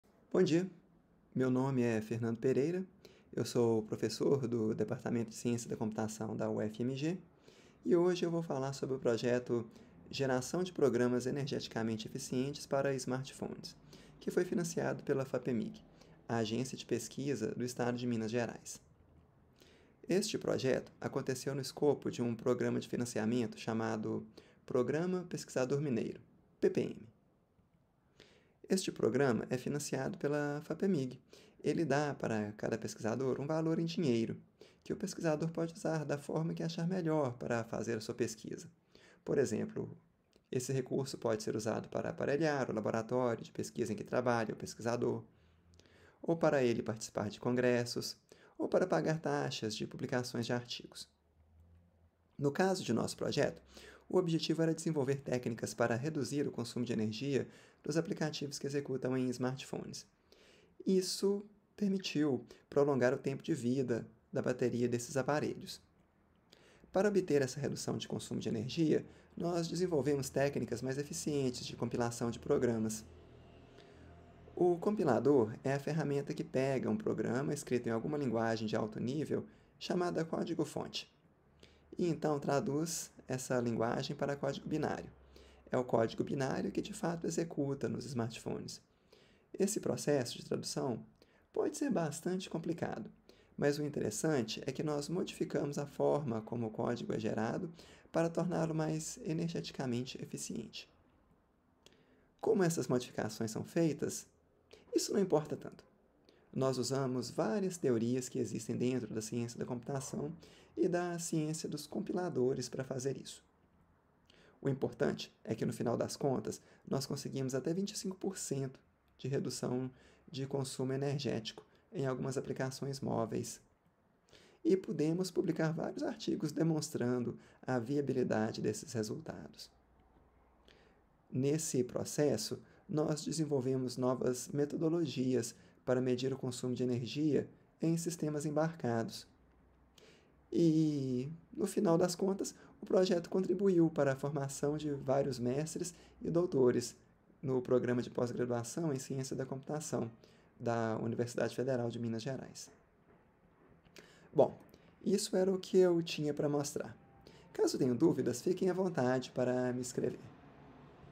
Geração de Programas Energeticamente Eficientes: Resumo de Projeto de Pesquisa.
Este video descreve o projeto "Geração de Programas Energeticamente Eficientes", que foi financiado pela FAPEMIG, no contexto do programa Pesquisador Mineiro.